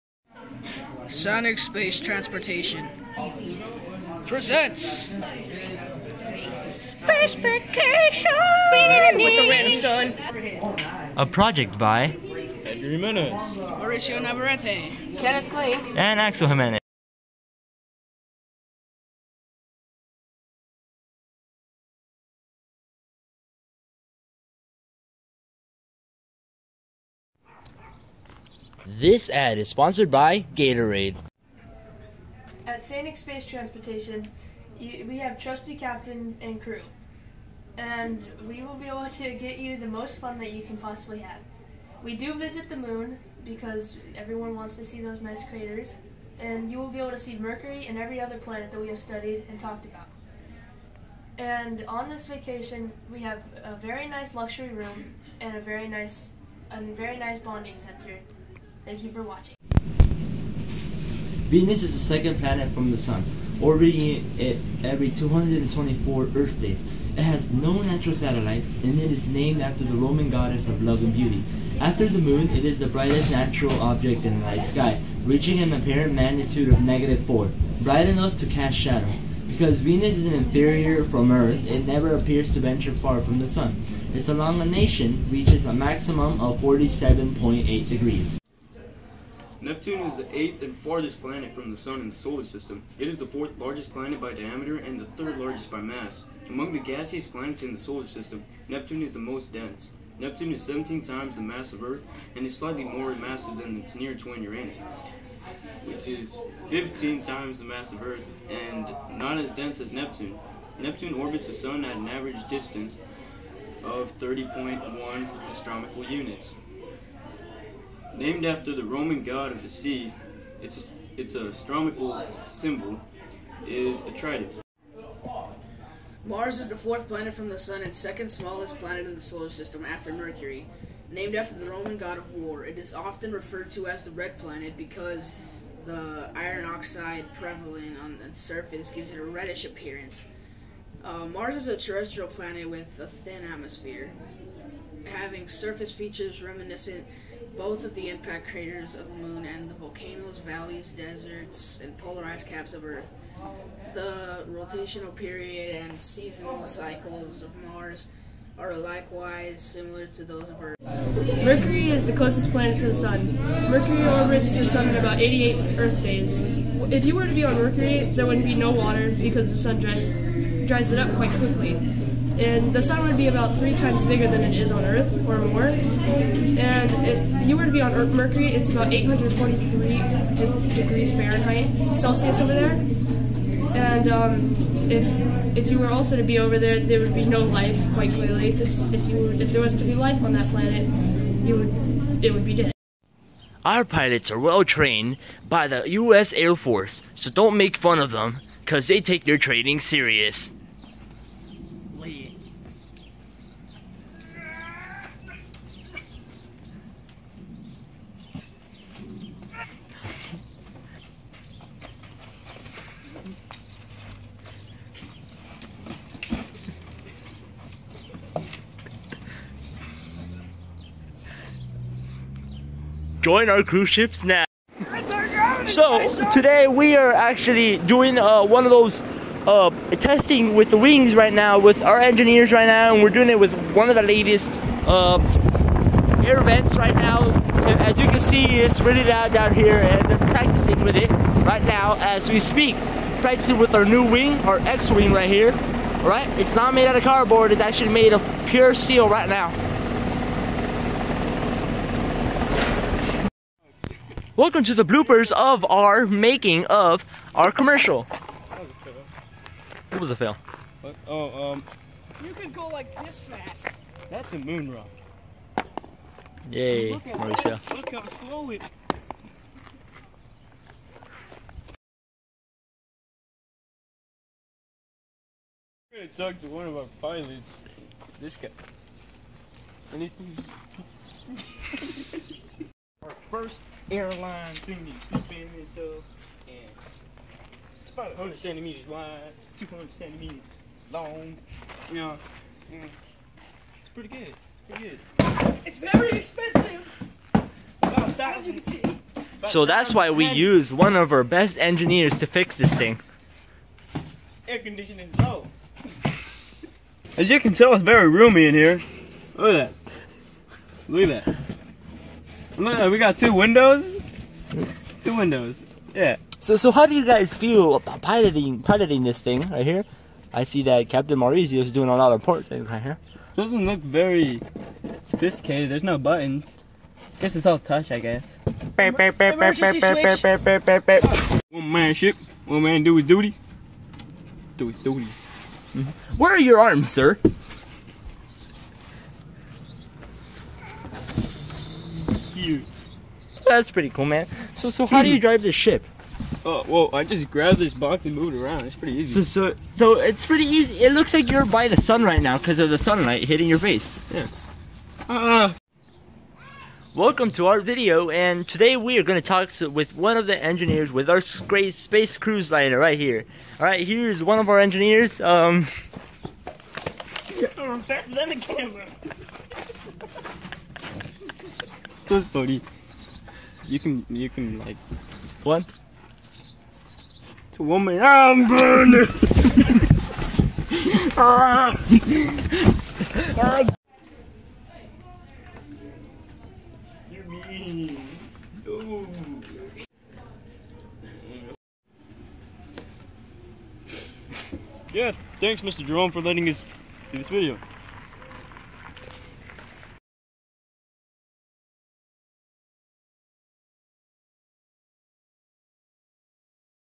SPACE VACATION
This is the first video i have ever edited and it was really fun doing this video for a project and i am looking forward to making more.
-Axel Jimenez, Greenway Middle School Student